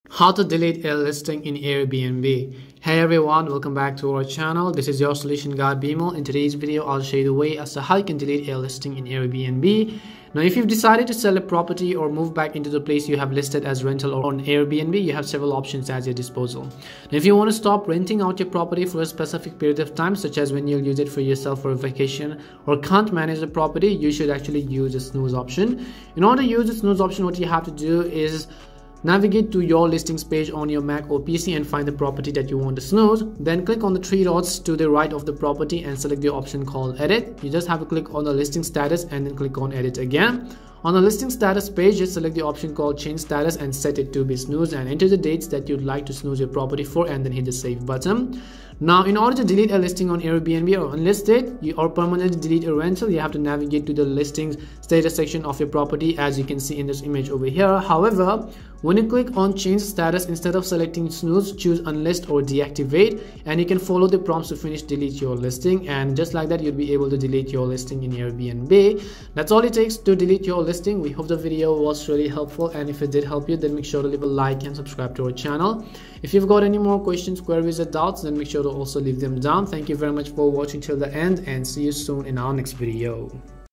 How to Delete a Listing in Airbnb !
In this tutorial video, I am simply going to show you how to delete a listing in Airbnb.

Follow these simple steps:
Go to the Listing Status section of your property, as shown in the video, to permanently delete a rental or to permanently unlist a property.

Instead of choosing sleep when you click "Change Status," pick "Unlist" or "Deactivate." To completely delete your listing, follow the instructions.

~Time Stamps:
0:00 Introduction
0:10 General Info
0:26 How to Snooze
0:48 How to Delete a Listing in Airbnb
1:12 Conclusion